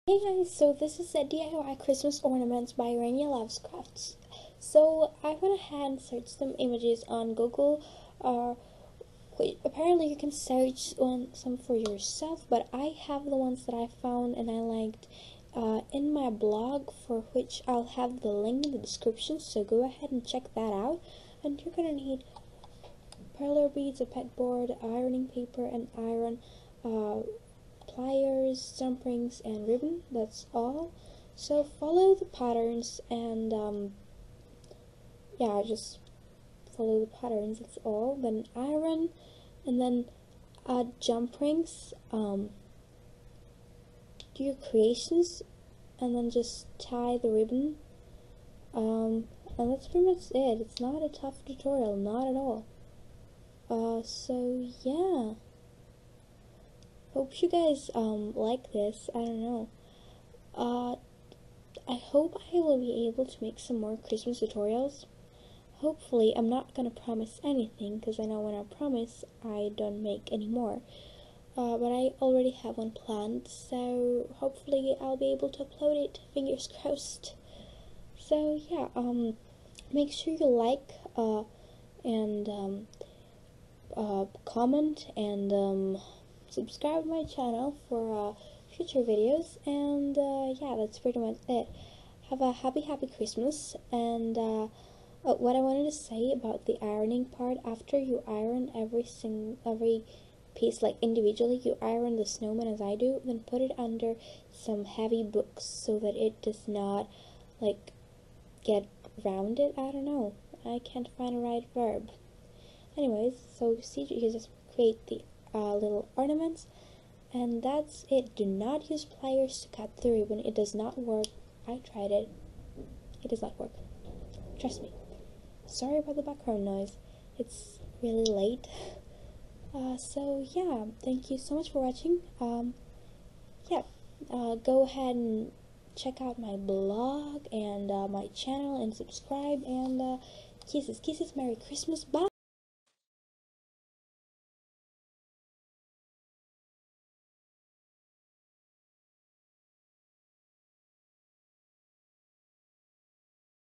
DIY: Perler beads Christmas Ornaments
Hello!!! Merry Christmas! Open for information

So that was just a really easy tutorial on how to make some really pretty christmas ornaments! And if you don't want to make it christmas looking you can always make other designs! Like summer ones! The sky is the limit guys!!! So go crazy with it!